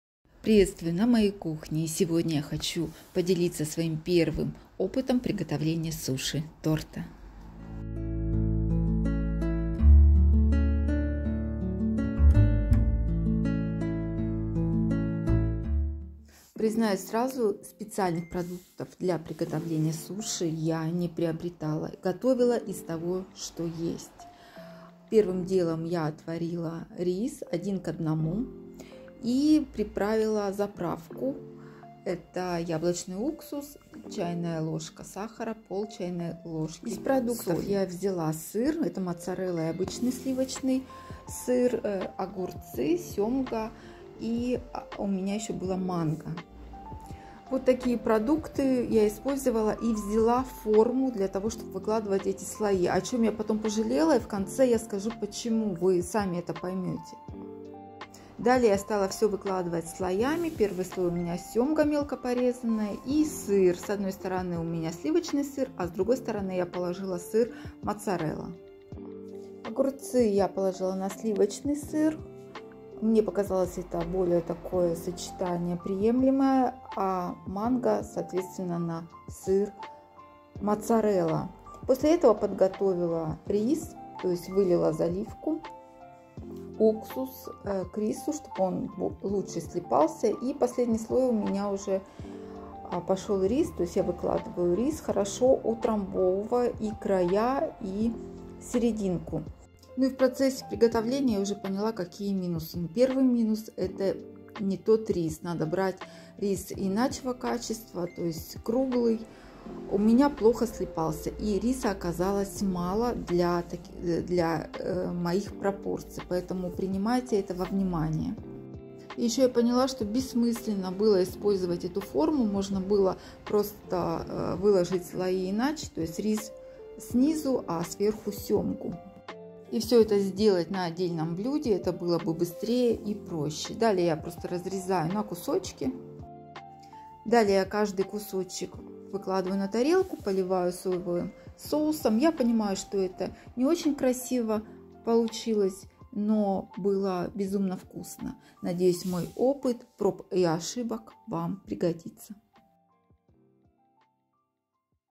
СУШИ-ТОРТ // Самый простой рецепт приготовления суши
Приветствую на моем канале!

В этом видео делюсь с вами моим первым опытом приготовления суши. Хотя получился суши торт. Хочу сказать, что несмотря ни на что, суши получились очень вкусными.

Я использовала примерное колличество:
200 гр. лосося
200 гр.риса
200 гр.воды
Сыр сливочный 100 гр
Сыр моцарела 100 гр
Манго 0.5 шт
Огурец 150 гр или 1/2 большого огурца
Уксус яблочный 15гр., 0,5 ч.л.соли, 1 ч.л.сахара

Приятного аппетита!